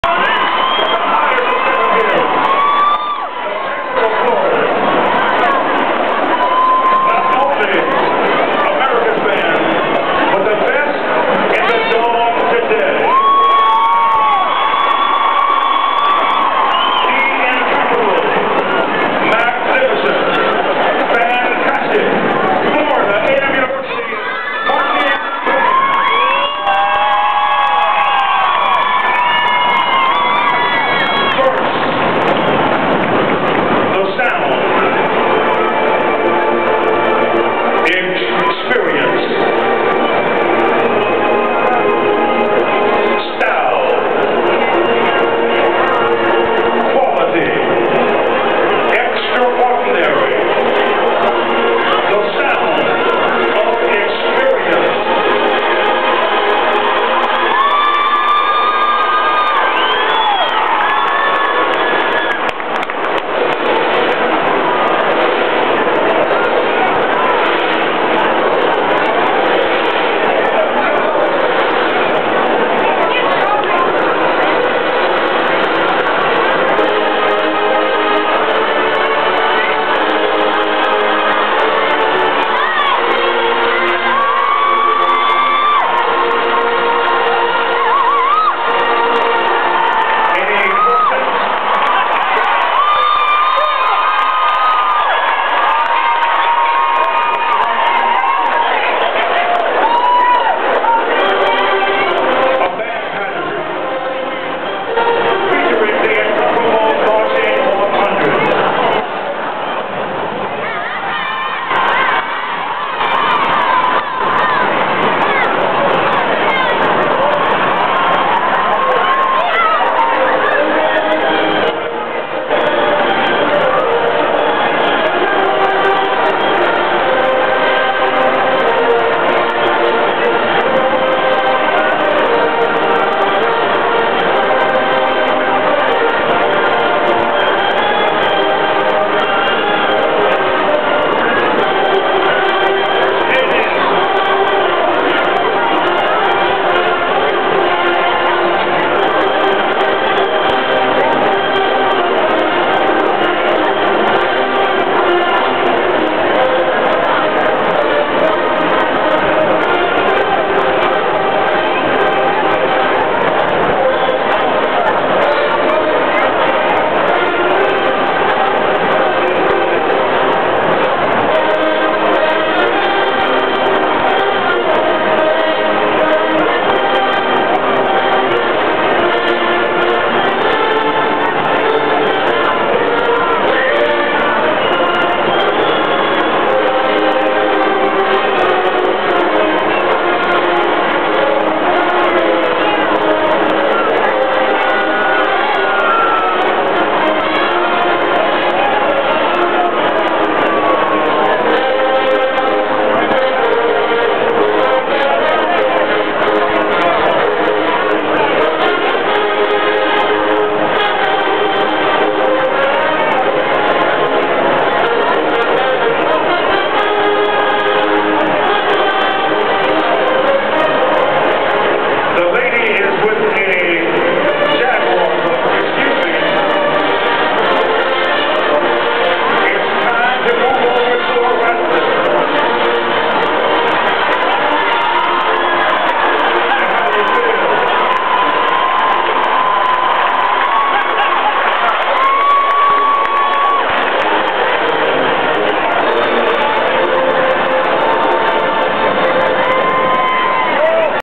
FLORIDA A&M UNIVERSITY MARCHING BAND @ 2011 ATLANTA FOOTBALL CLASSIC!!!
Earlier today, the FLORIDA A&M UNIVERSITY (FAMU) RATTLERS defeated the SOUTHERN UNIVERSITY (SU) JAGUARS 38-33 at the 2011 ATLANTA FOOTBALL CLSSIC at the GEORGIA DOME in The ATL. FAMU took the field second in the legendary halftime BATTLE Of The BANDS. Here is the first part of the Halftime Show from the FLORIDA A&M UNIVERSITY MARCHING BAND. -CCG